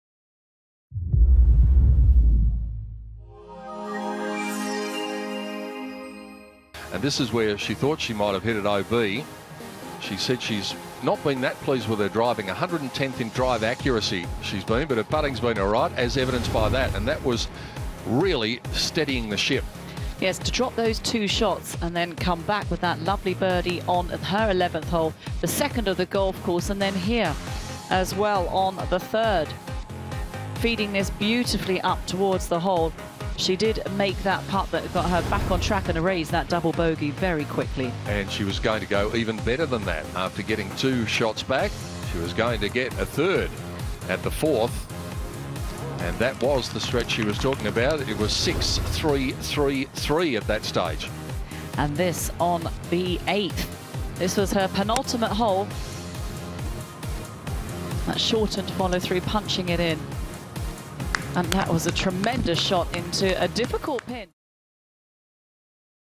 Su Oh First Round Highlights - First Round 2017 Sime Darby LPGA Malaysia
Su Oh cards a first round 65 to put her just one stroke back from the leader.